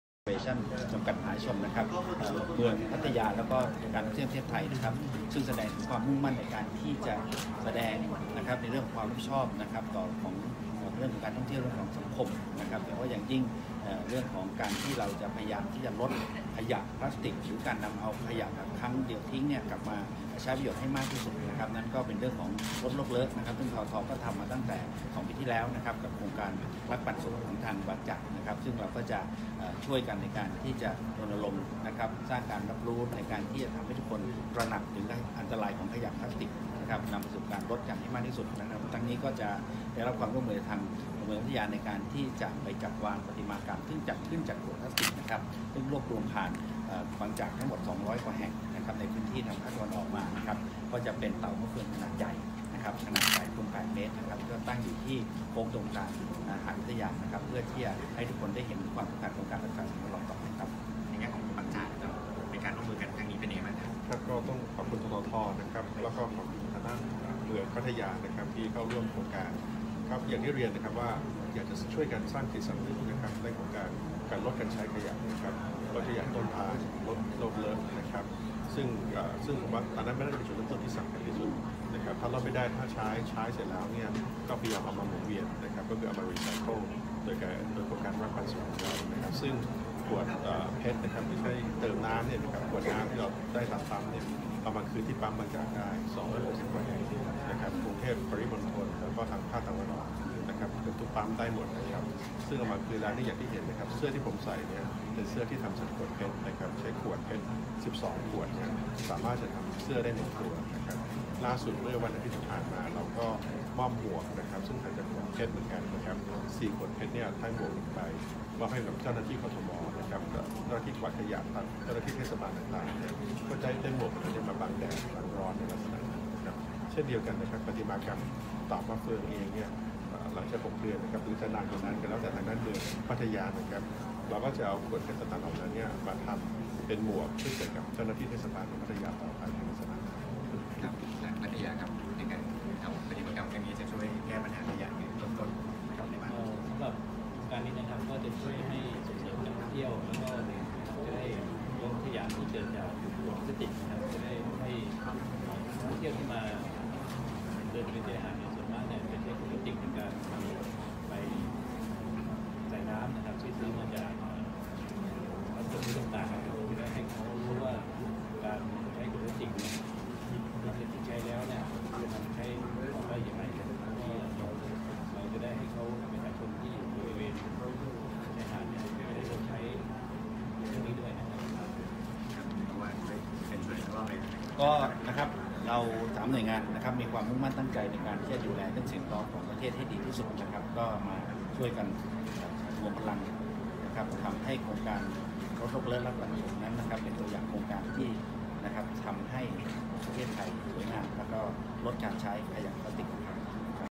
บางจากทุ่ม 800 ล้าน ขยายปั๊ม 60 แห่ง ปี 63
บางจากอัดงบ 800 ล้านบาทขยายสถานีบริการน้ำมันเพิ่มอีก 60แห่งในปีหน้า เผยโรงกลั่นจ่อขยายกำลังการผลิตเพิ่มเป็น 1.3 แสนบาร์เรล/วันได้ในไตรมาส 4/63 หลังปริมาณน้ำมันที่กลั่นได้ไม่เพียงพอขายผ่านปั๊มน้ำมันแล้วจนต้องนำเข้าเบนซิน